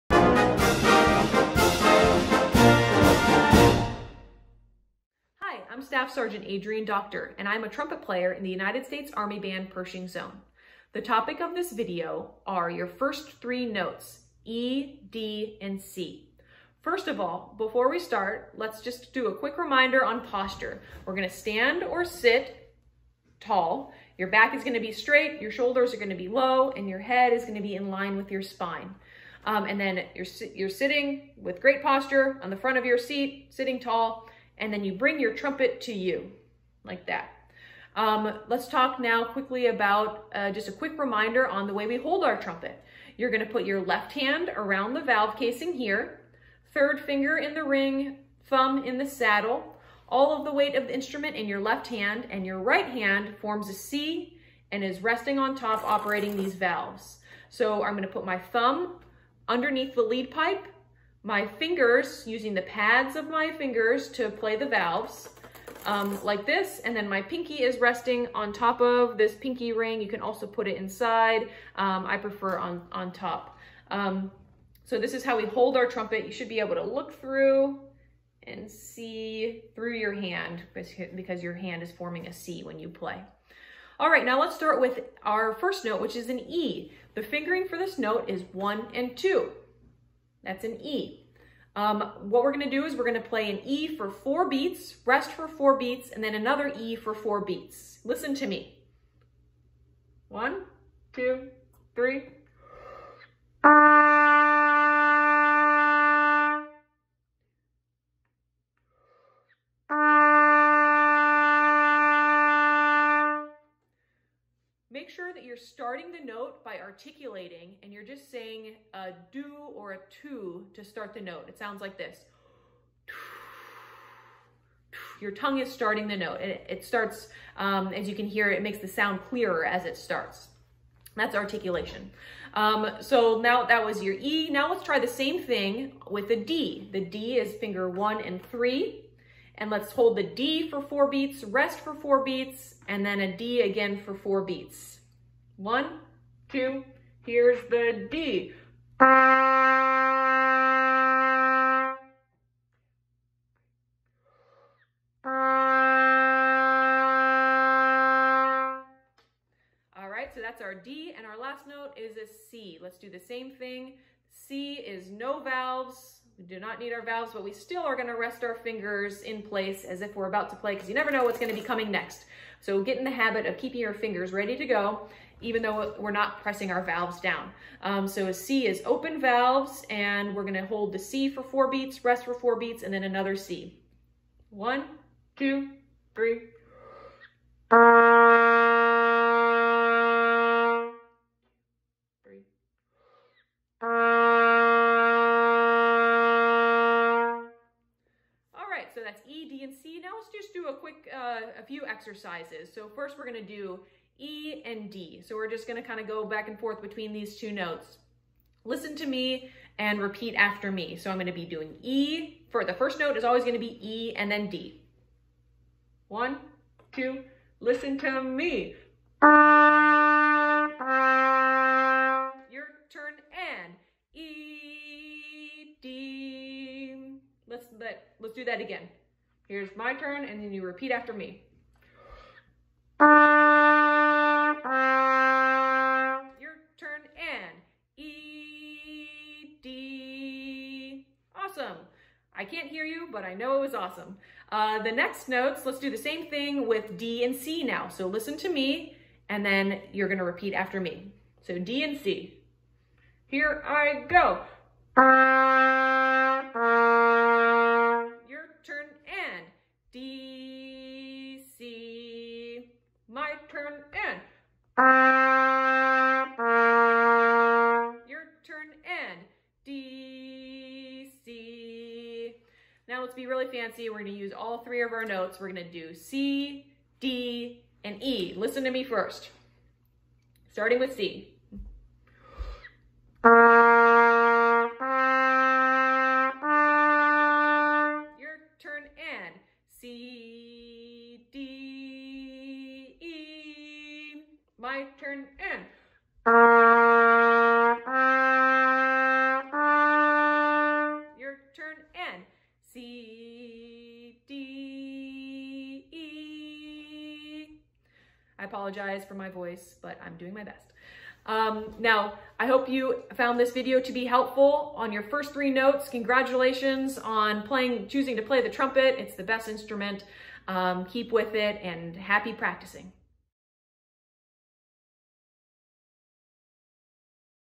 Beginner Trumpet Series: First Three Notes- E, D, C
In this video, SSG Adrienne Doctor teaches trumpet students learn how to play the first three notes on the instrument: E, D, & C.